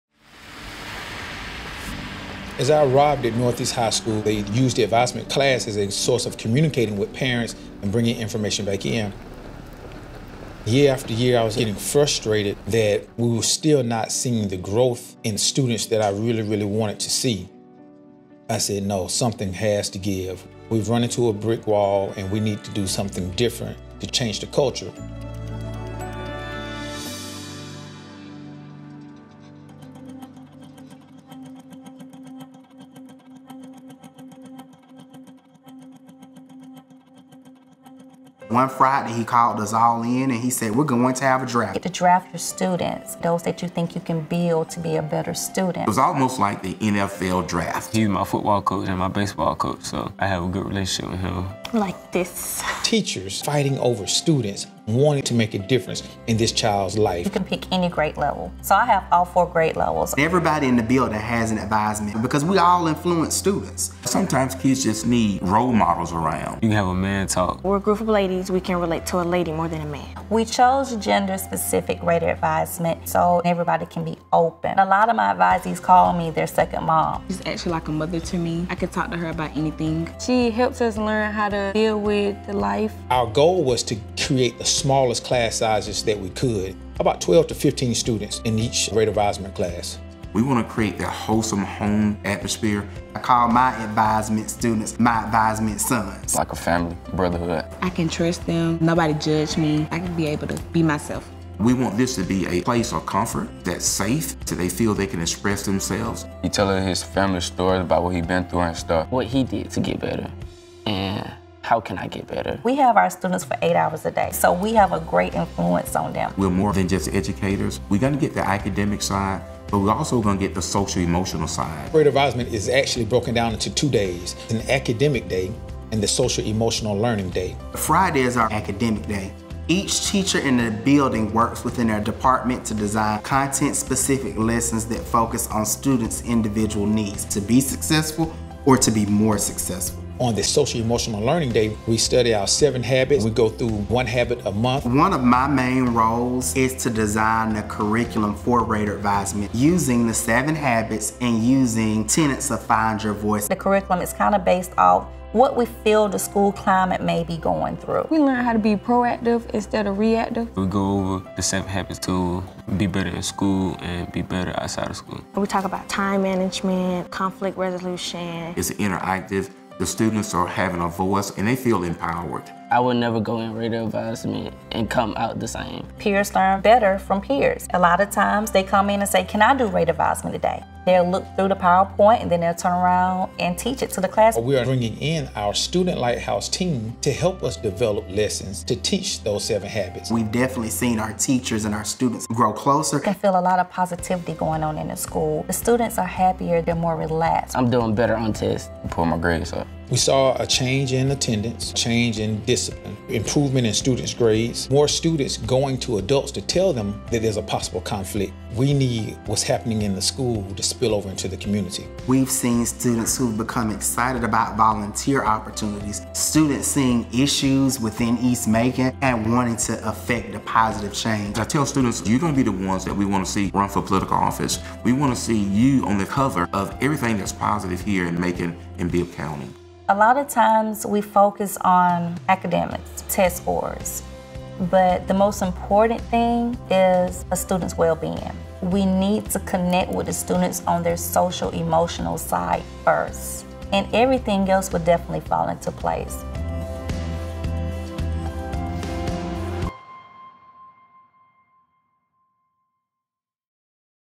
Northeast High School's Raidervisement
Northeast High School in Macon, Ga., implemented Raidervisement periods to focus on supporting the whole child. Teachers and staff "draft" students to their teams at the beginning of the school year and they focus not only on academic improvement, but also on supporting the social and emotional well-being of students.

"We saw a change in attendance, a change in discipline, improvement in students’ grades, more students going to adults to tell them that there’s a possible conflict. We need what’s happening in the school to spill over into the community," Principal R. Steven Jones said.

Credit: FranklinCovey